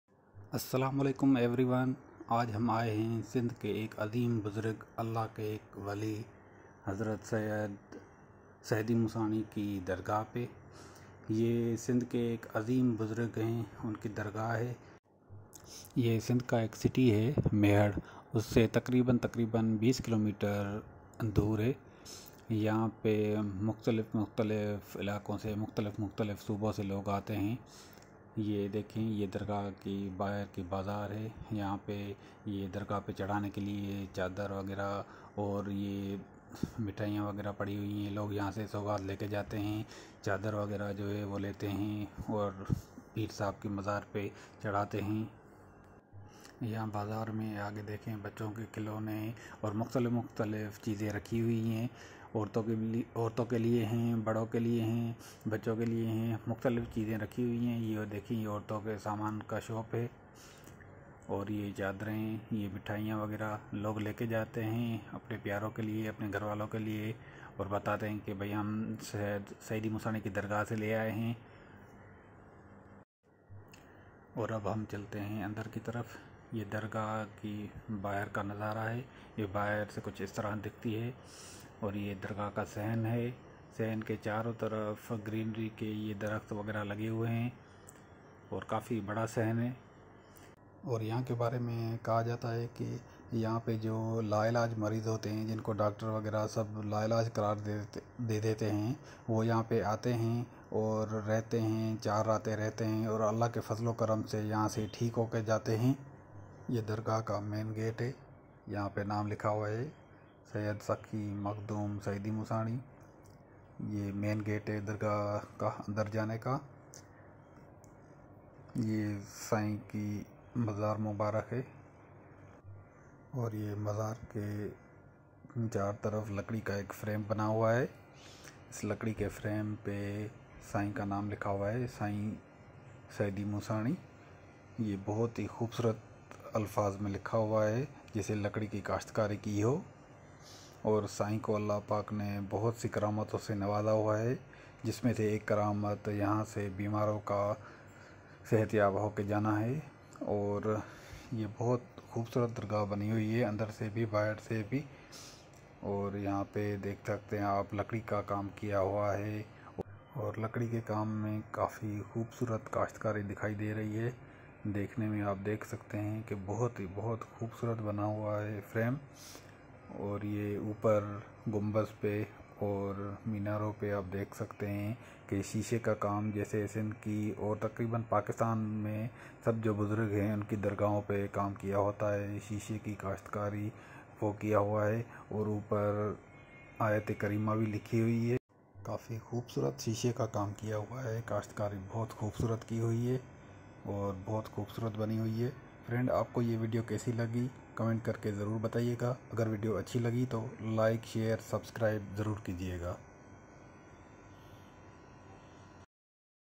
visit to the dargah of syed sayedi moosani at mehar sindh
Assalamuallaikum everyone
visit to the dargah of syed sayedi moosani